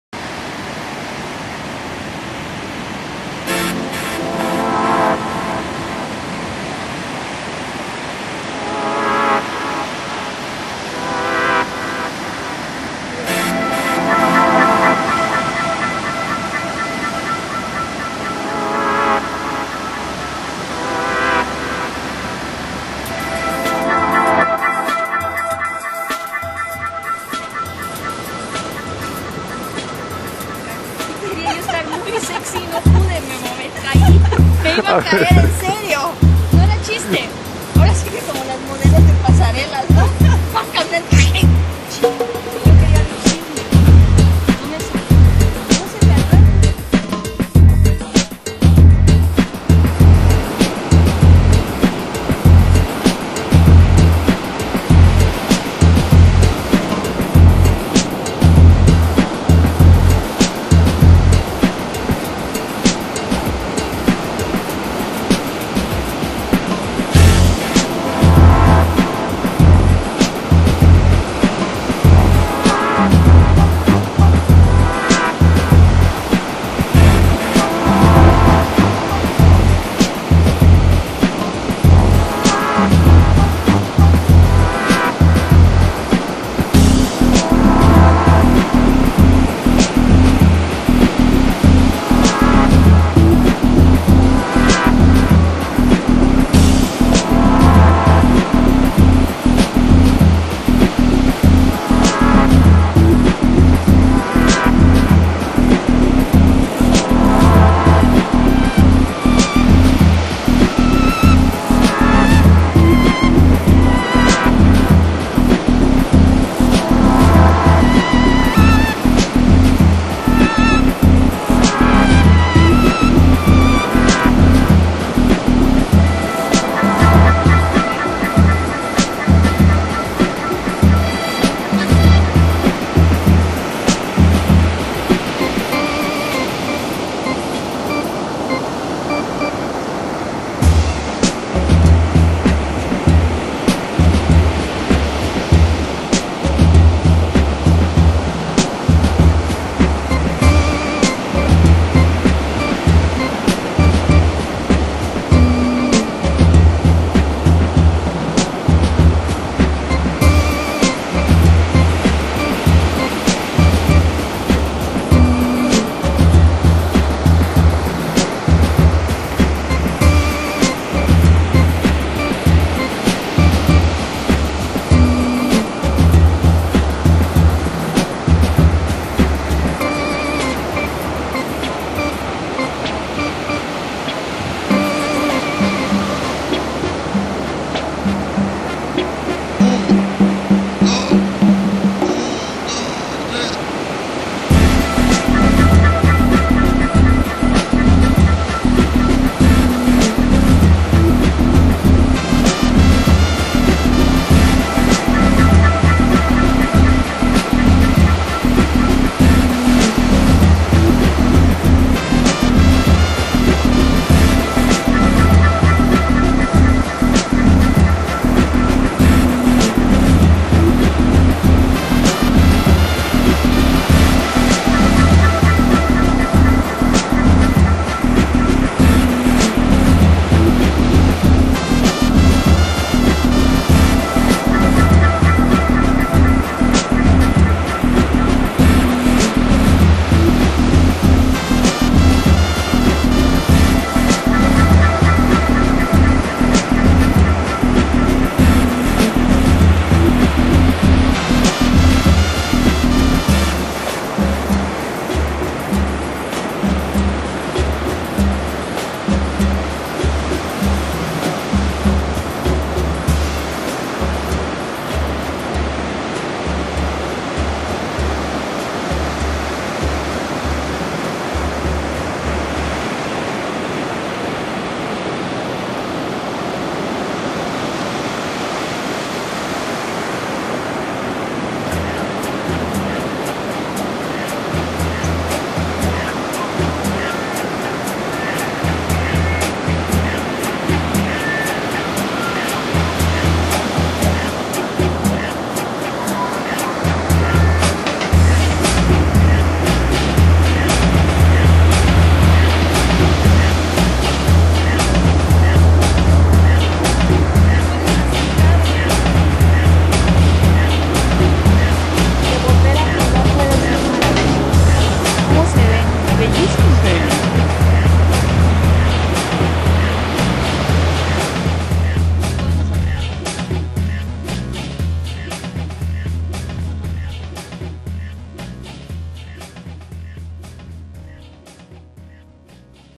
El mar y las Barcas de Tere
En la Playa de Costa Esmeralda Tere expone sus barcas que por primera vez se van a la Mar.